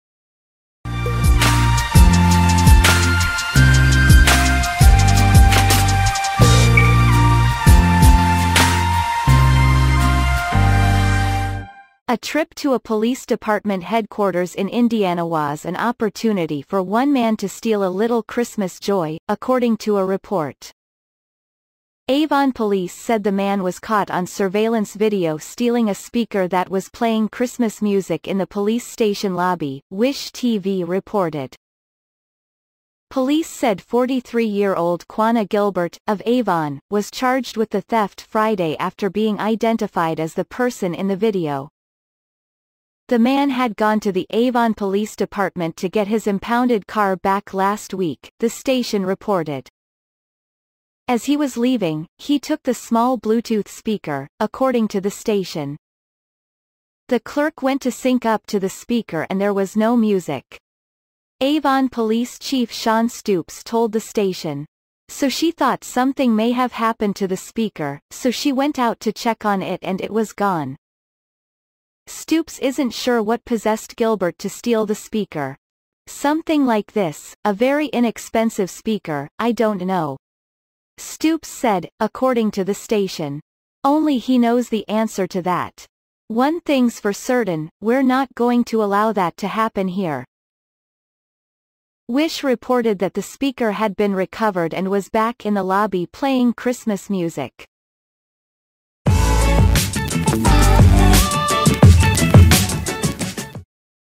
Indiana man stole police department speaker playing Christmas music, cops say
A trip to a police department headquarters inIndianawas an opportunity for one man to steal a little Christmas joy, according to a report.  Avon police said the man was caught on surveillance video stealing a speaker that was playing Christmas music in the police station lobby, WISH-TV reported.  Police said 43-year-old Quannah Gilbert, of Avon, was charged with the theft Friday after being identified as the person in the video.  The man had gone to the Avon Police Department to get his impounded car back last week, the station...